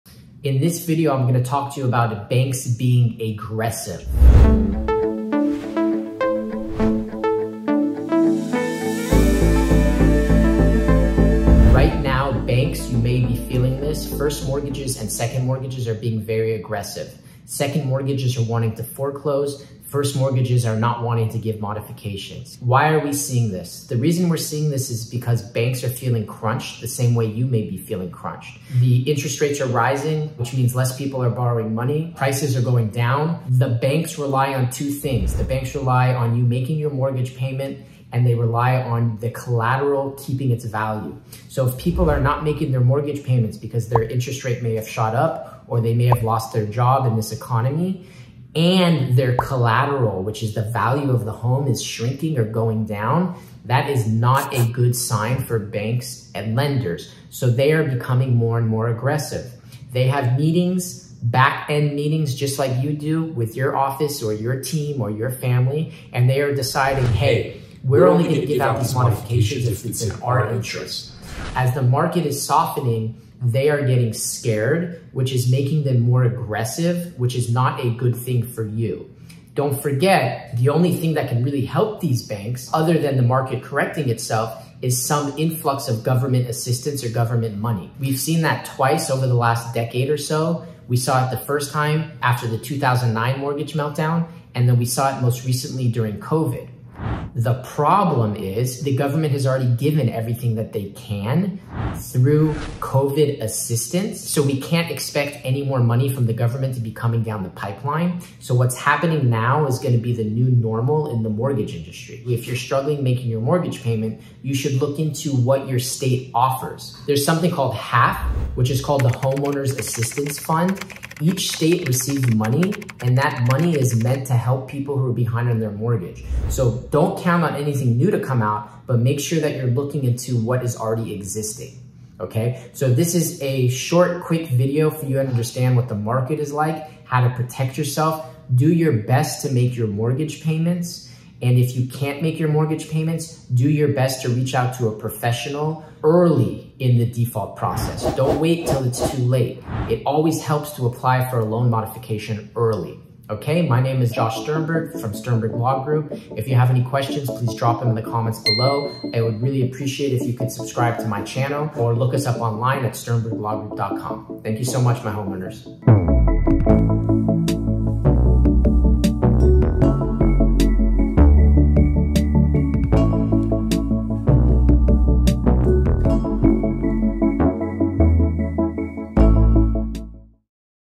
Why BANKS are getting Aggressive NOW?
Are you struggling with your Mortgage Payments?
In this video, I will talk about "Why are Banks getting aggressive in 2023?"

Timestamp
00:00 Banks are getting Aggressive
00:23 Why are banks getting aggressive?
00:35 The banks rely on 2 things
01:19 The only thing that's gonna help the banks
01:48 What should you do if you're struggling to make your mortgage payments

Would you like to learn more about our Sternberg Law Group?

USEFUL RESOURCES